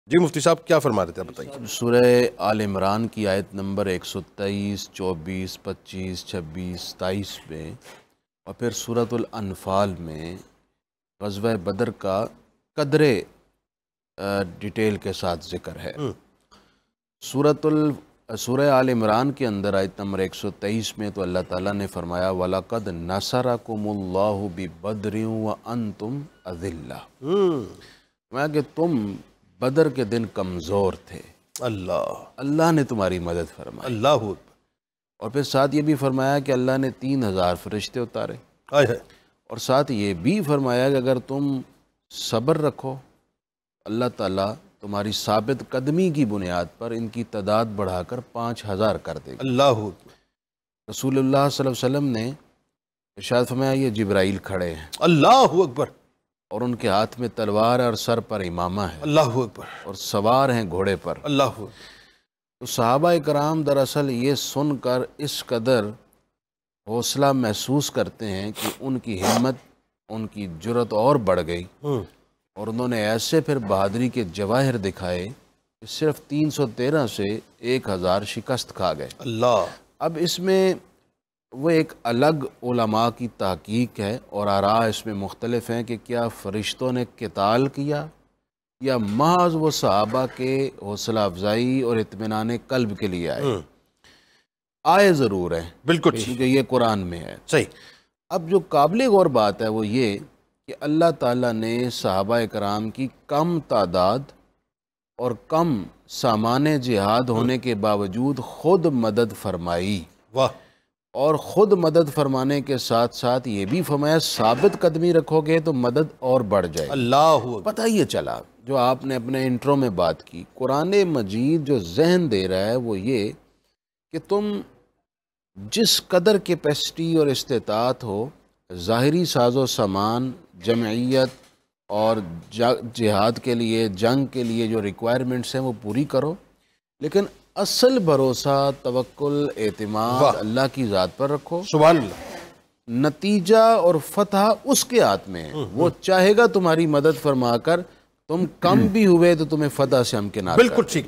Significance of Battle of Badar in Holy Quran | Exploring Battle of Badar | 28 March 2024 | 92NewsHD
Significance of Battle of Badr in Holy Quran | Exploring Battle of Badar | 28 March 2024 | 92NewsHD